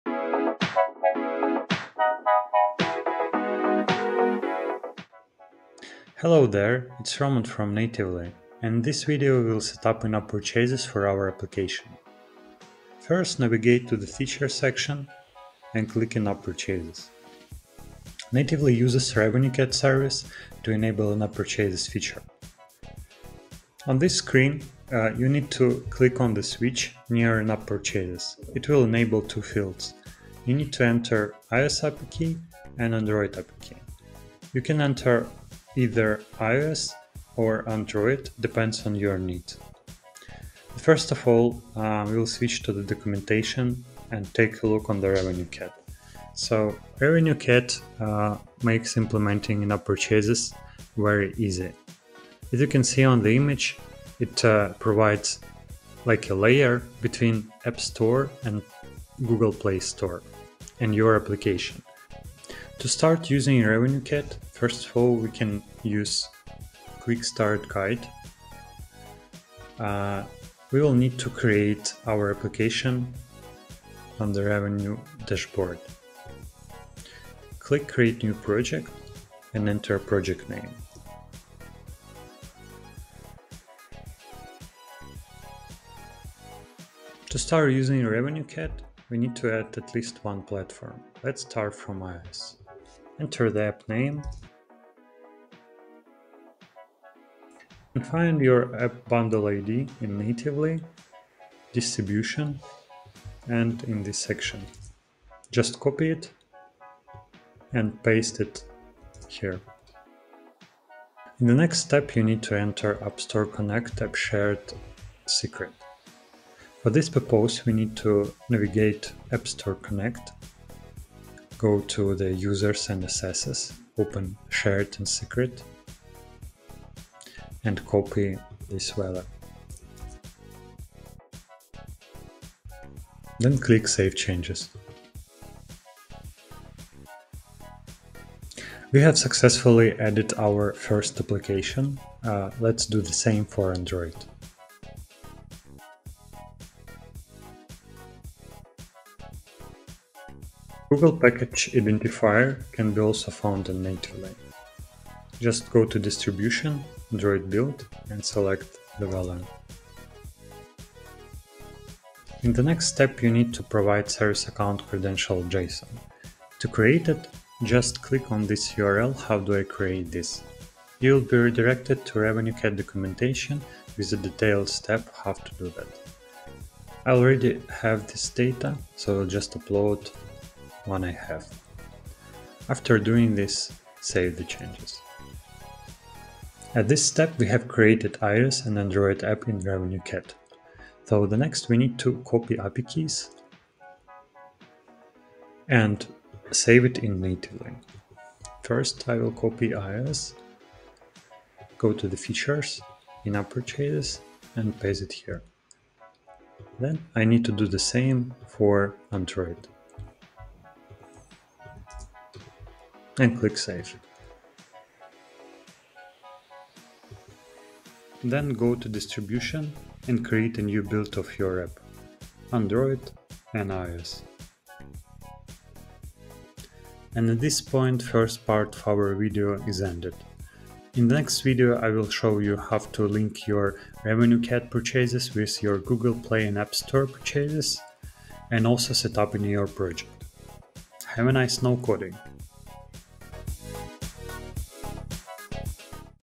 Natively 2.0 How to set up in app purchases (Part 1)
In-app purchases puts your sales at the customer's fingertip, almost quite literally.

See how Natively allows you to set up in-app purchases using RevenueCat service.

with ❤️ from 🇺🇦